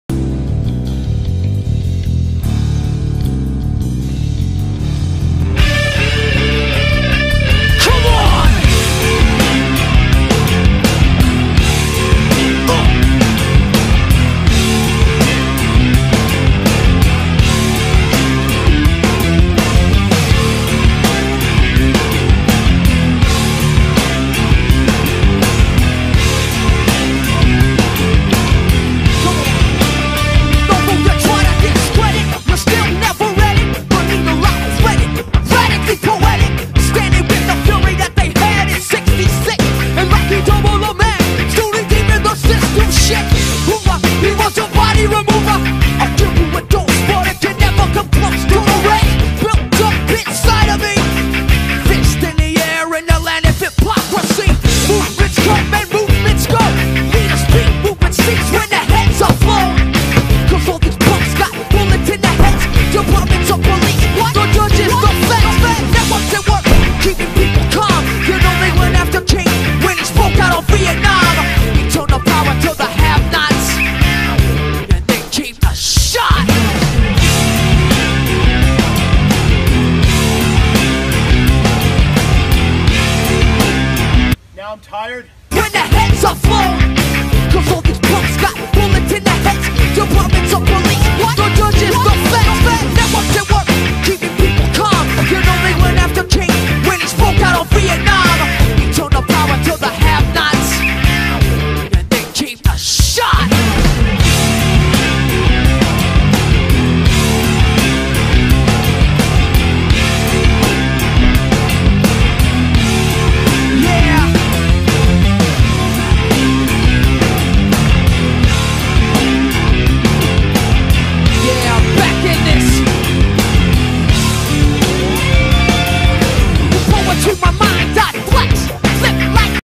DefenseLite Window Protection and Glass Security
See how DefenseLite holds up against repeated attacks using blunt objects such as a hammer or a baseball bat.